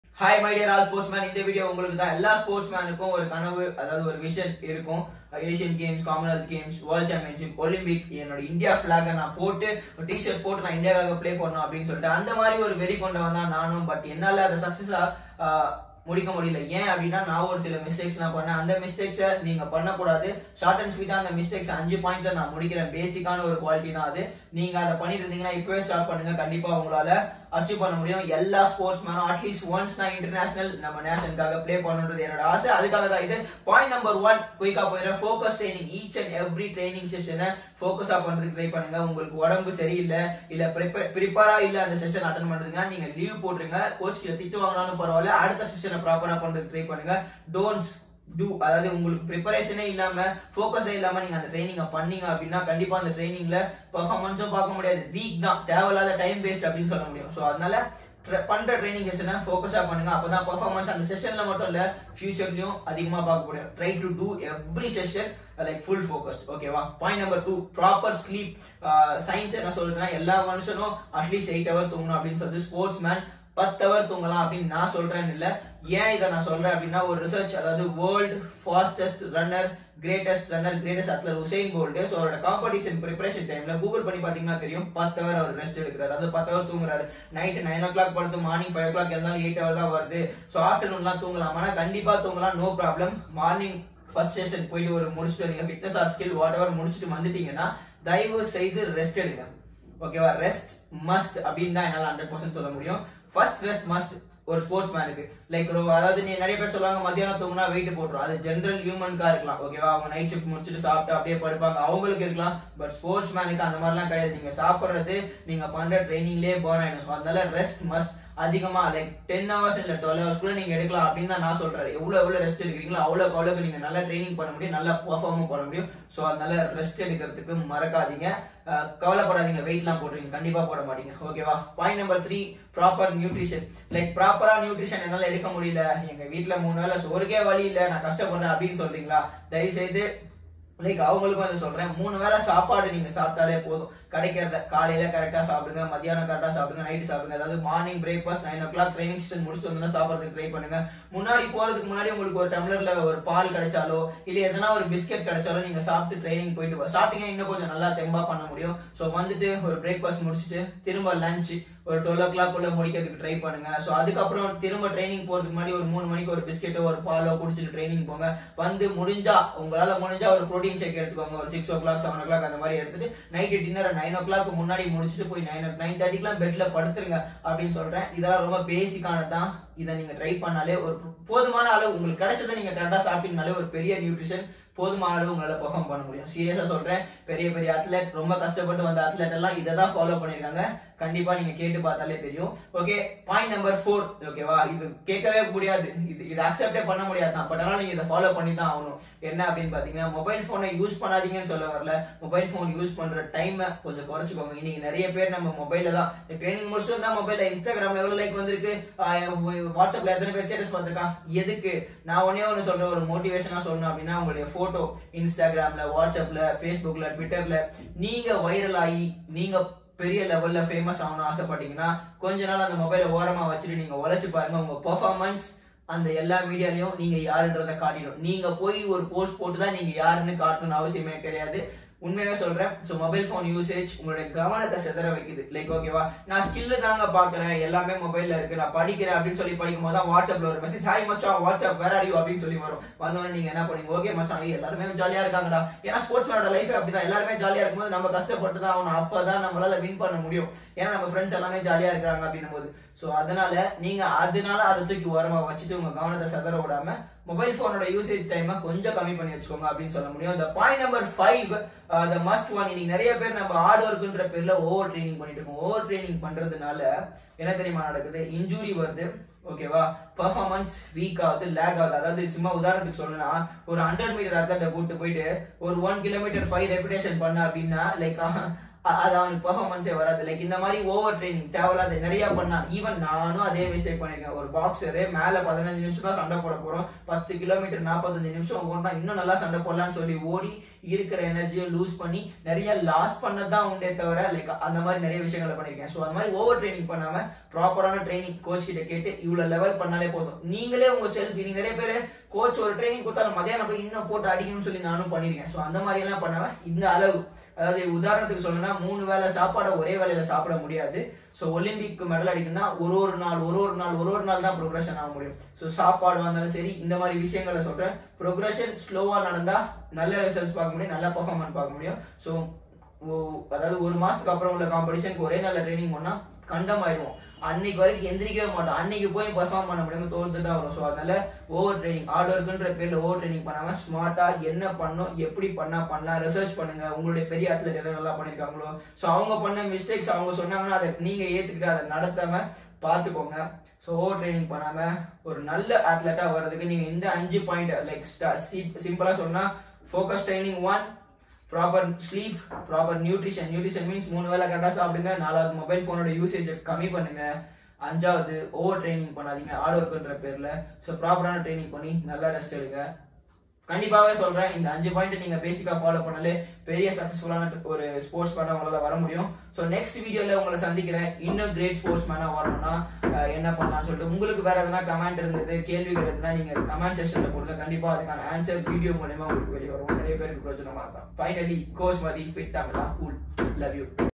Sportsman’s &Athlete இந்த video உங்களுக்குதான் watch -Follow this easy 5 step then achieve you Goals
🏆 FOLLOW THIS BASIC 5 POITS 🏆
1. Focus 🧘‍♂️ training
2. Proper  sleep 🛌
3. Proper nutrition 🥘
4. Cut time mobile phone usage
5 . Stop over training